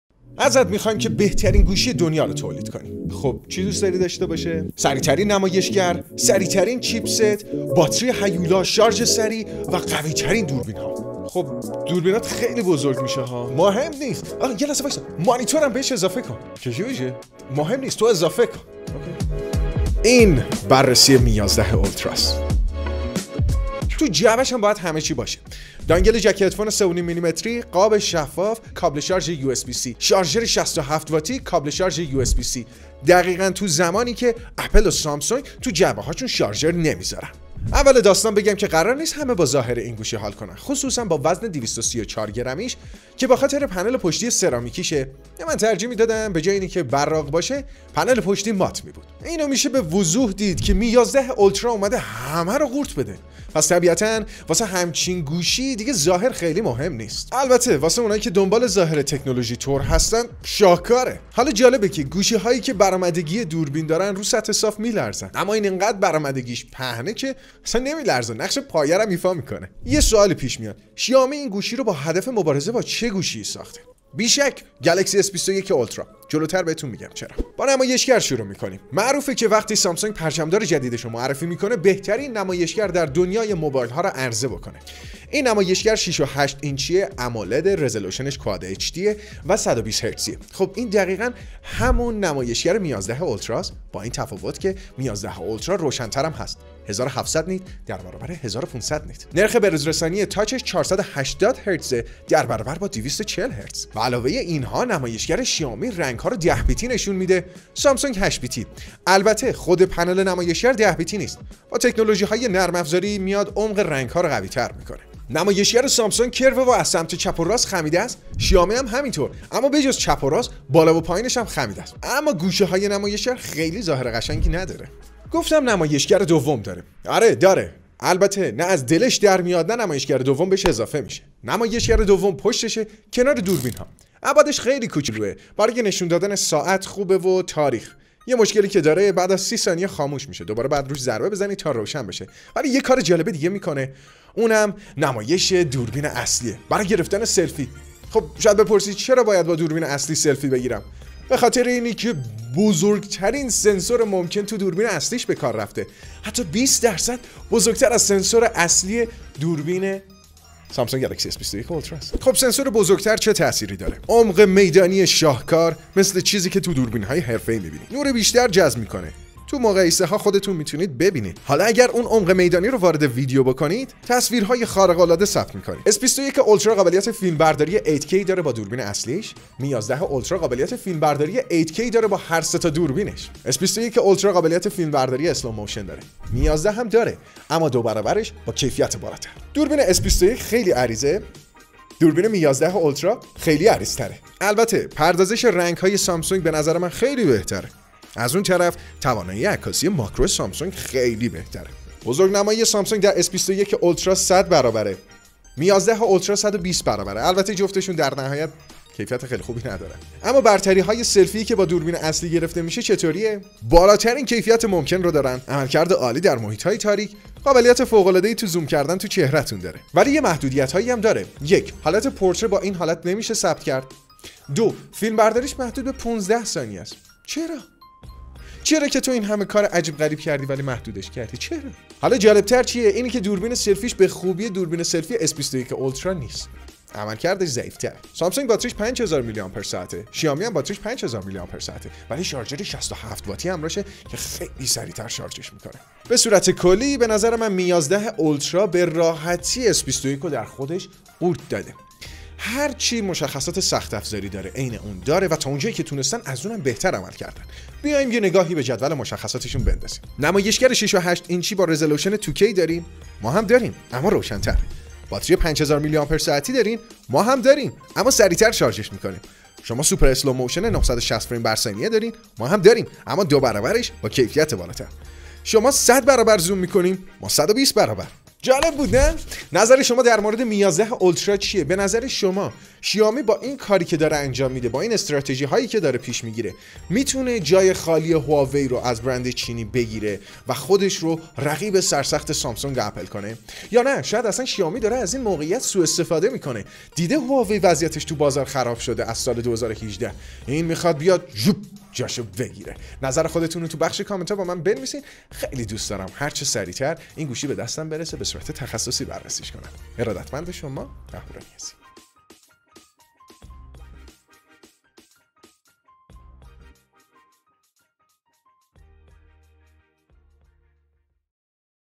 من قاتل گوشی هام | بررسی شیائومی می ۱۱ اولترا
Some Major Part of Video does not Belong to me
شیائومی با خالی بودن رقیب بزرگ همزبانش یعنی هواوی طی ۳ سال گذشته حسابی از فرصت استفاده کرده و با سرمایه گذاری های سنگین
 در زمینه گوشی های موبایل، دست به نوآوری های خوبی زده و شهرت و جایگاه خوبی در بازار بدست آورده
می ۱۱ اولترا  نام آخرین پرچمدار شیائومی هست که با امکانات جذاب خودش میتونه لقب برترین گوشی موبایل رو از نظر امکانات به خودش اختصاص بده
رقیب سر سخت اون سامسونگ گلکسی اس ۲۱ اولتراست، ویدیو رو ببینید و نظر بدید آیا تونست از سامسونگ برتری داشته باشه؟
بخشی از مشخصات کلیدی اون :
نمایشگر اول ۶.۸ اینچی امولد ۱۰ بیتی رزلوشن ۲کی و نرخ بروز رسانی ۱۲۰ هرتزی نرخ بروز رسانی تاچ ۴۸۰ هرتزی و بیشینه روشنایی ۱۷۰۰ نیتی
نمایشگر دوم  ۱.۱ اینچ
چیپست اسنپرداگون ۸۸۸،  ۸ تا ۱۲ گیگابایت حافظه رم، ۲۵۶ تا ۵۱۲ گیگابایت حافظه داخلی
دوربین اصلی ۵۰ مگاپیکسلی (بزرگترین سنسور گوشی های موبایل )، دوربین تله فوتو ۴۸ مگاپیکسلی، دوربین خیلی عریض ۴۸ مگایپسکلی
باتری ۵۰۰۰ میلی آمپر ساعت همراه با شارژ سریع ۶۷ واتی
لینک زیر شما رو سابسکرایب میکنه
ممنونم که با لایک ، کامنت و سابسکرایب کردن کانال از من حمایت میکنید
 صفحه ایسنتاگرامم : shorturl.at/nHN18
صفحه توییترم : shorturl.at/qAPZ1
صفحه شخصیم تو اینستاگرام : shorturl.at/movB0